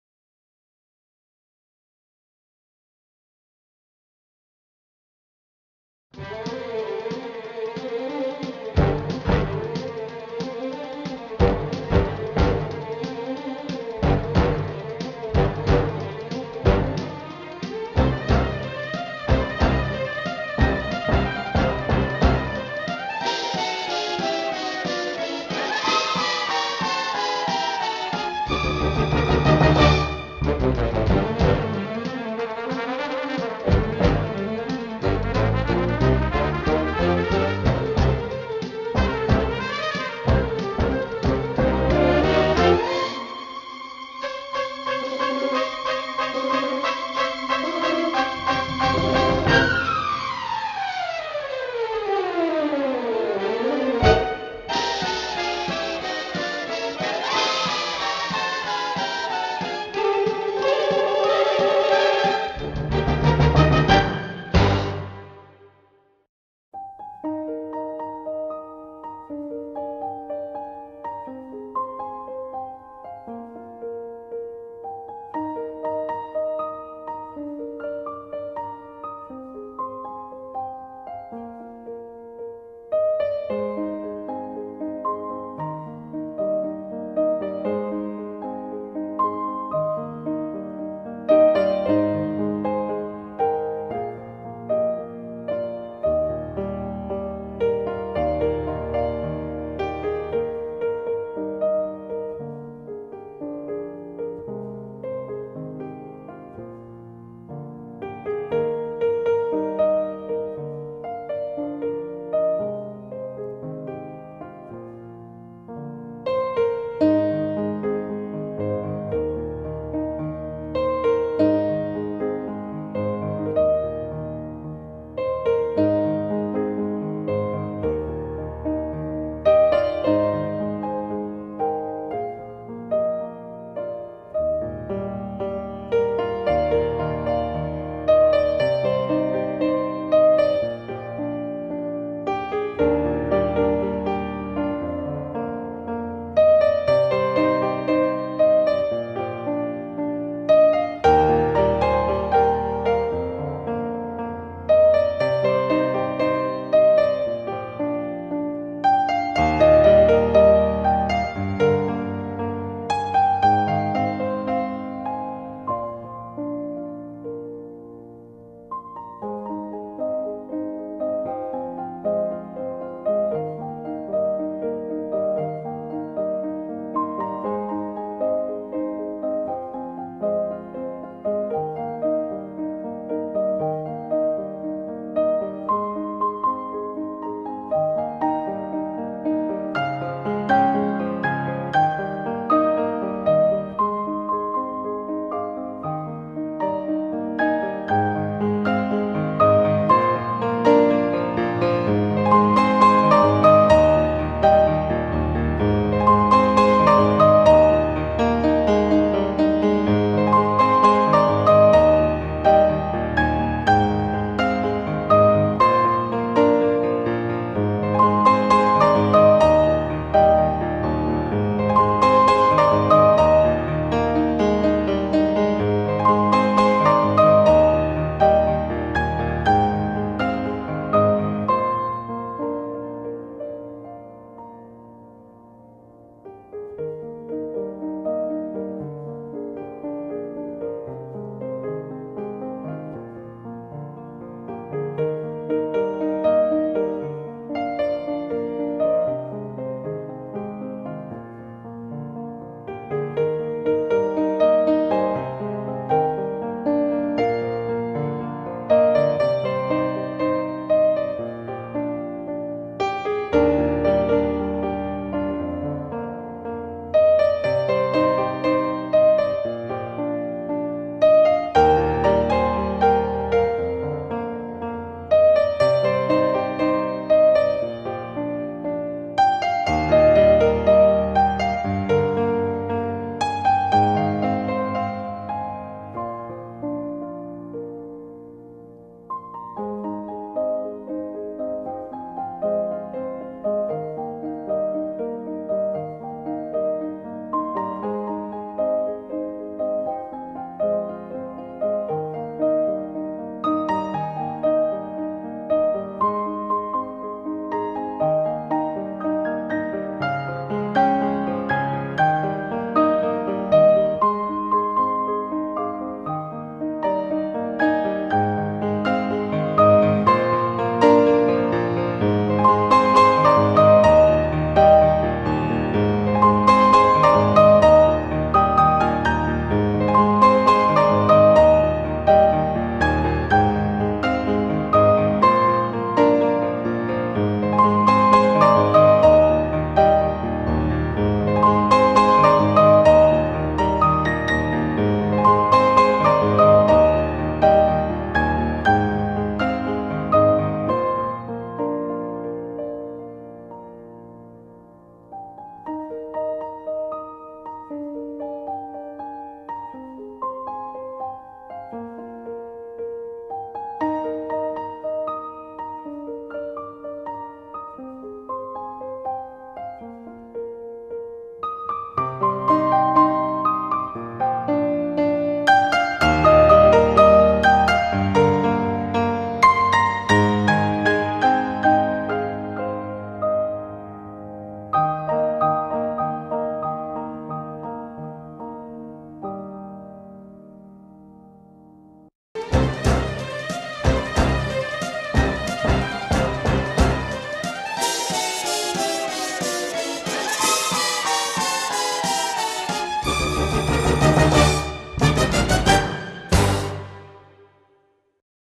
Mayflies and Orb Weaver Spiders on the Lewis and Clark Bridge
The Lewis and Clark Bridge in Louisville has a great deal of insect activity in the Summer. Mayflies swarm on the last two days of their lives and Orb Weaver Spiders are prepared to do their part in the cleanup.  Summer of 2018 both were abundant.
Music licensed Thru Presentation Audio and Flying Hands.